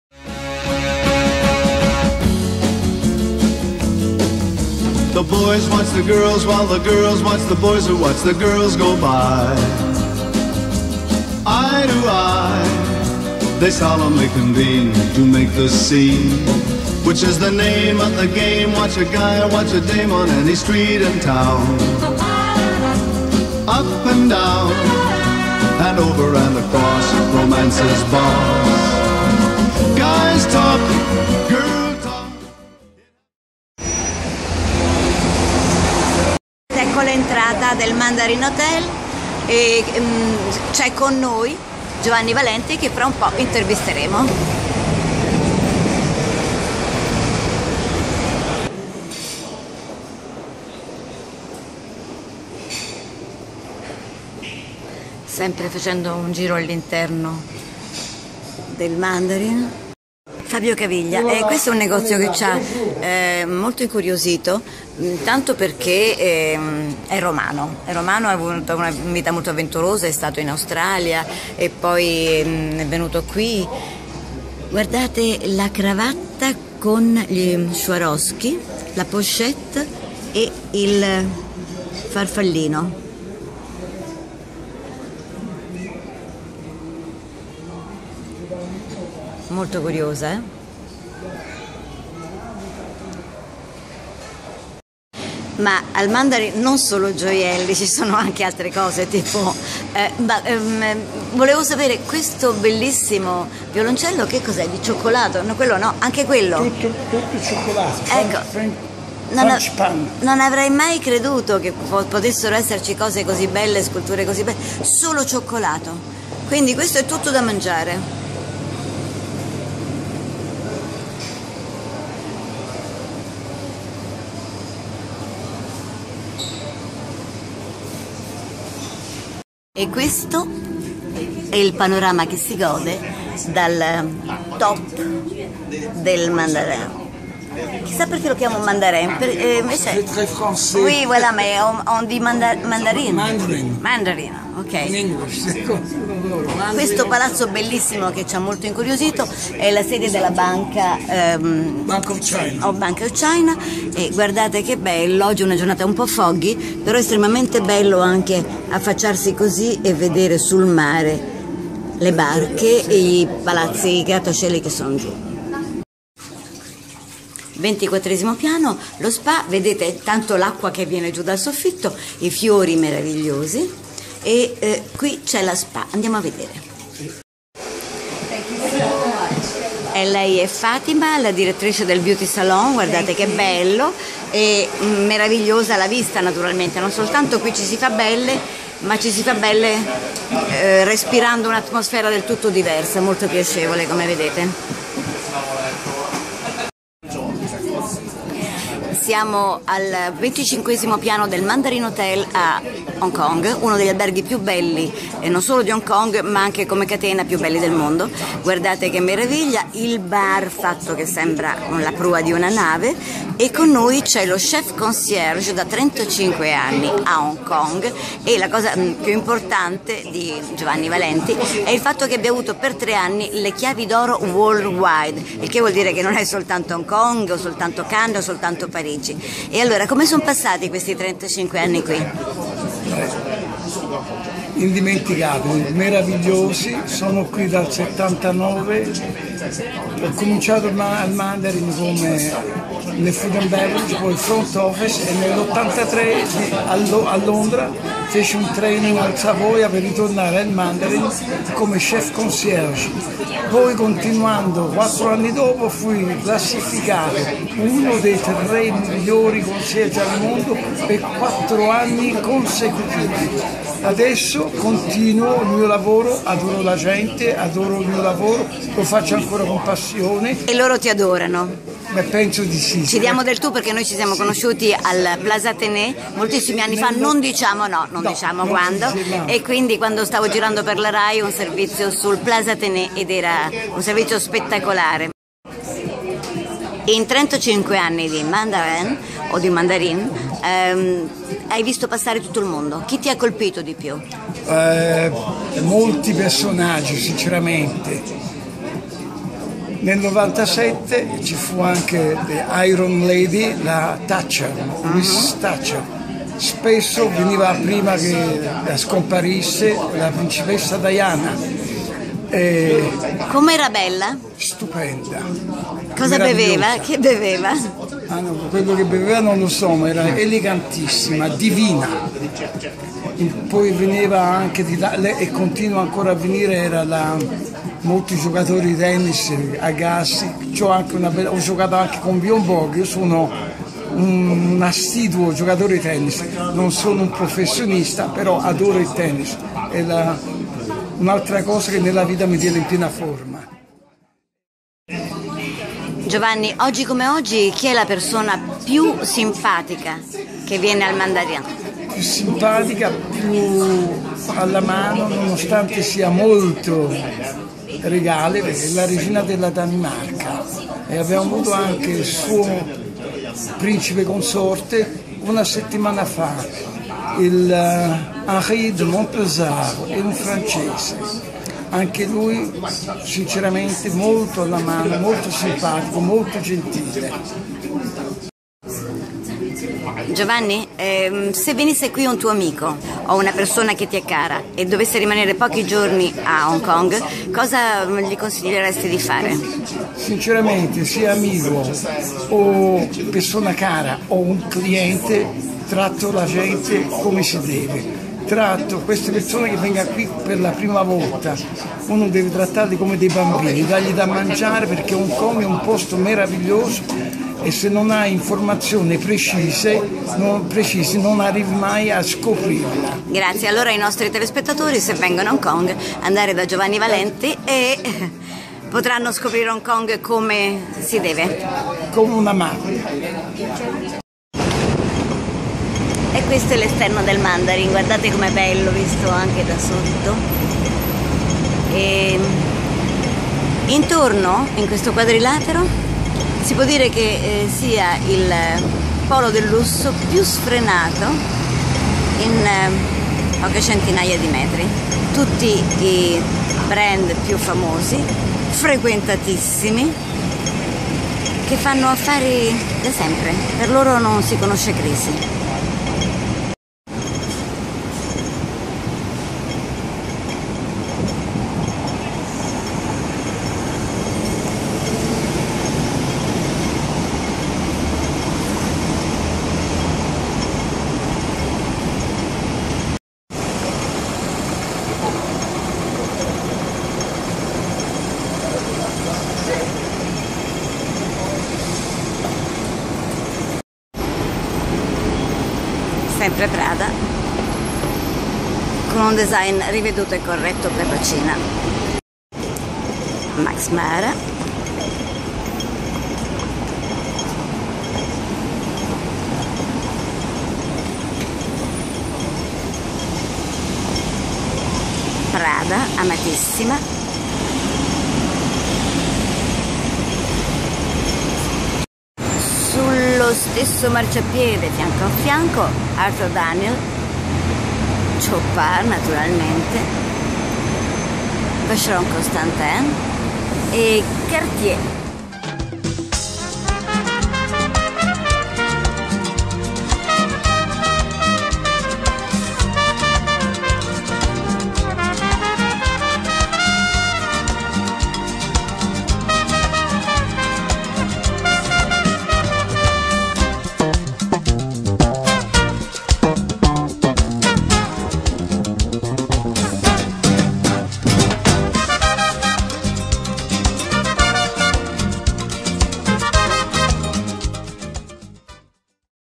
Intorno al Mandarin Oriental di Hong Kong il polo del lusso
Servizio e riprese di Mariangiola Castrovilli per VisumTv